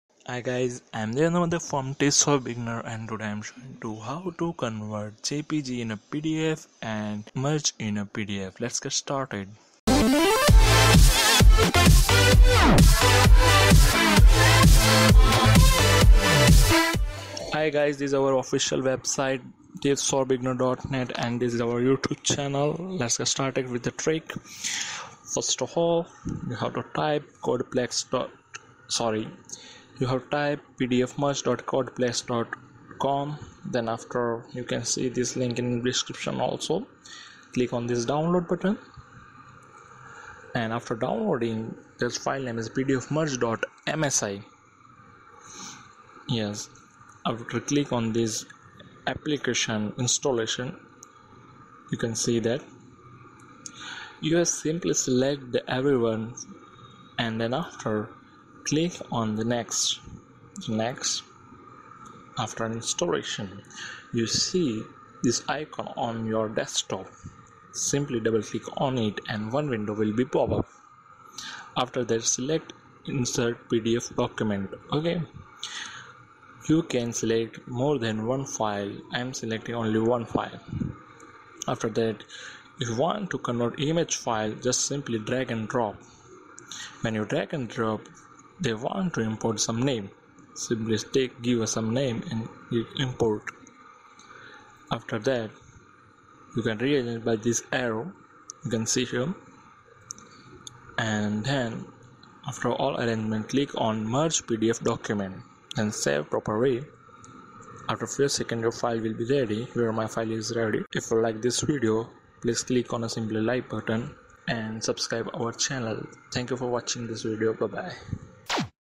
how to merge PDF files into one PDF and convert jpg (any file) to PDF
Convert image, jpg, png, gif, icon, word, text, jpg into pdf , no need photoshop, merge pdf files into one free
Benefits:
→ convert jpg to pdf
→ convert images to pdf
→ merge pdf files into one pdf
→ rearrange files in pdf
→ free